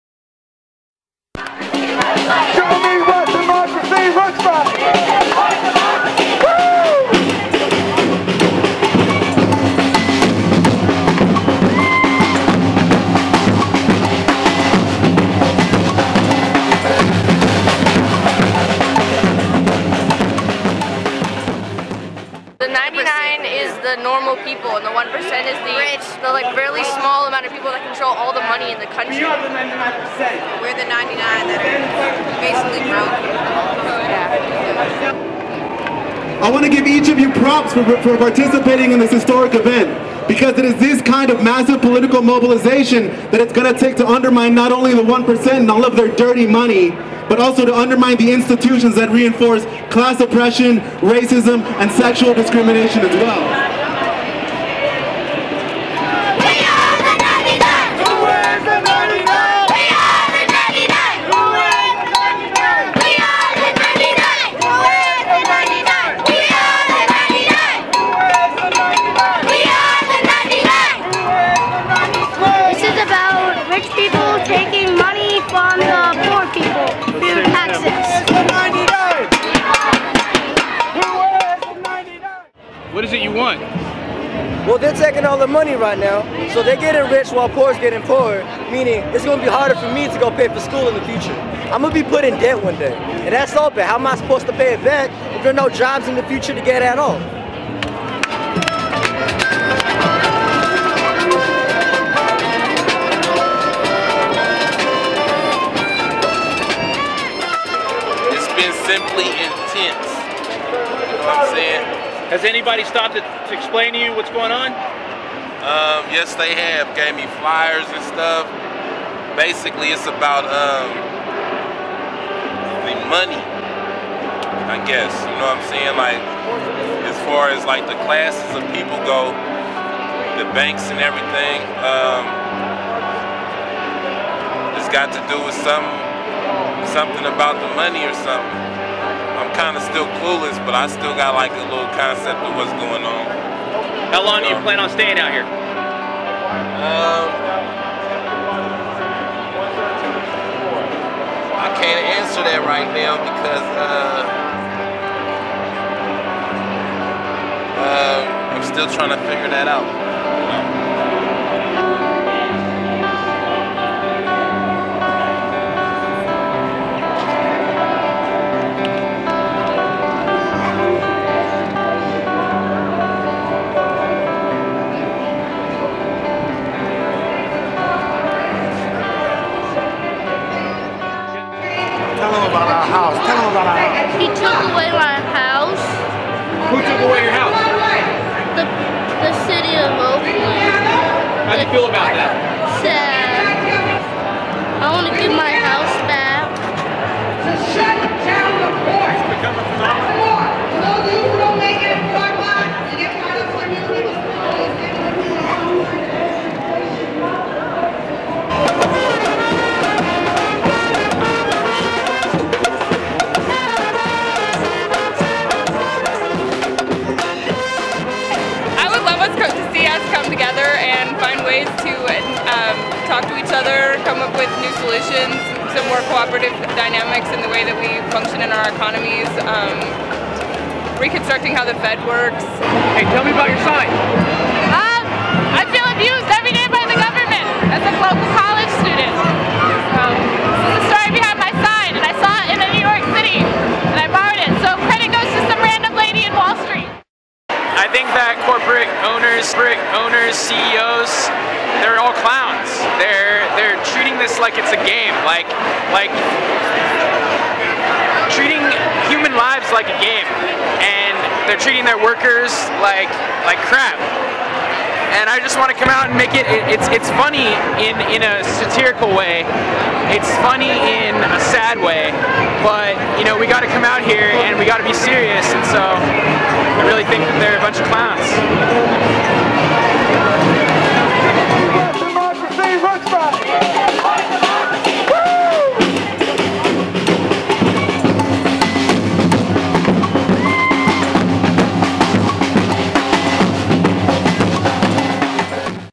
Occupy Oakland
Mario Sevilla, Bay Area News Group, takes you to Frank Ogawa Plaza to talk with Occupy Oakland protesters for their side of the story.